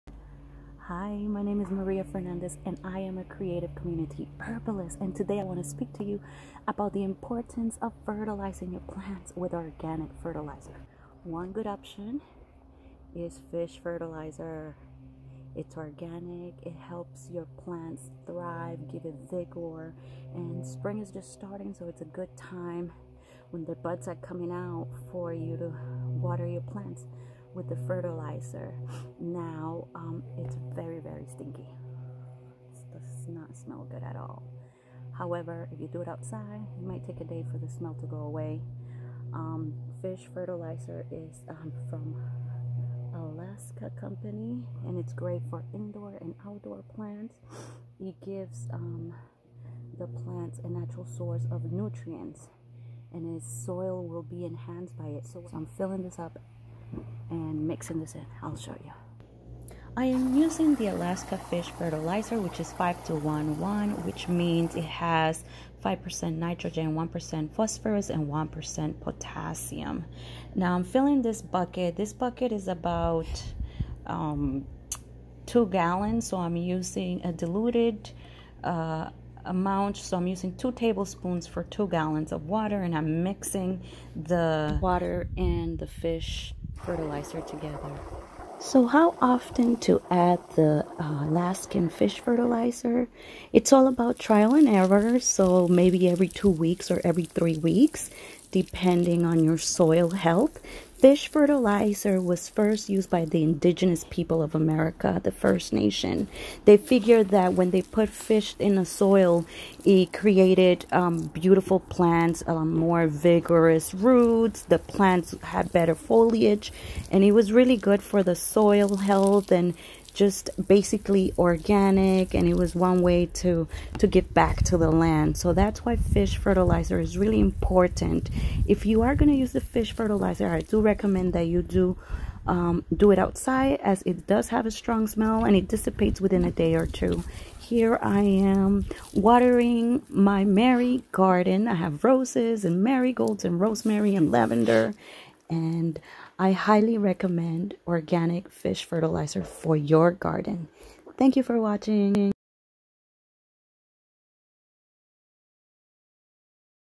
HOW TO USE ALASKA FISH FERTILIZER 🐠
🐟HOW TO USE ALASKAN FISH FERTILIZER 🐠

🌿On this video I talk about the benefits of using organic fish fertilizer.

🌿Plants needs not only need water, air, sunshine, love and communication. They also need to be fed plant food, which is why feeding them fish fertilizer is important on top of adding compost and granular organic nutrients.

🌿Your plants will be larger and will produce more flowers and fruit. They'll also be better able to fend off pests and disease, and will have better resistance to drought and other weather stress.